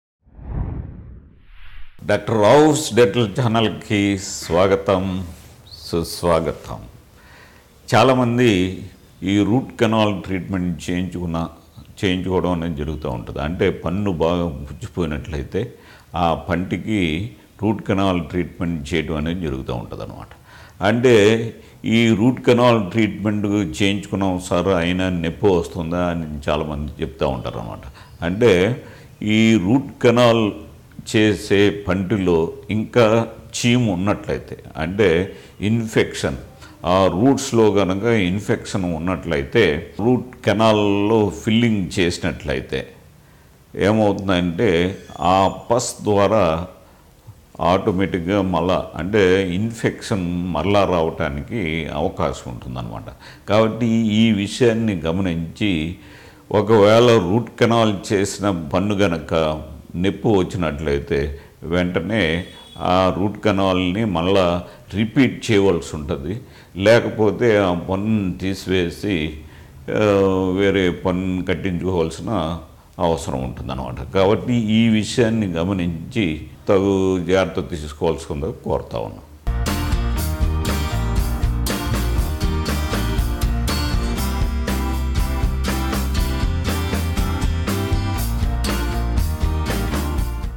రూట్ కెనాల్ ట్రీట్ మెంట్ చేయించుకున్న కూడా పంటి నొప్పి వస్తుందా | Root Canal Treatment
WATCH : రూట్ కెనాల్ ట్రీట్ మెంట్ చేయించుకున్న కూడా పంటి నొప్పి వస్తుందా | Root Canal Treatment  | Root Canal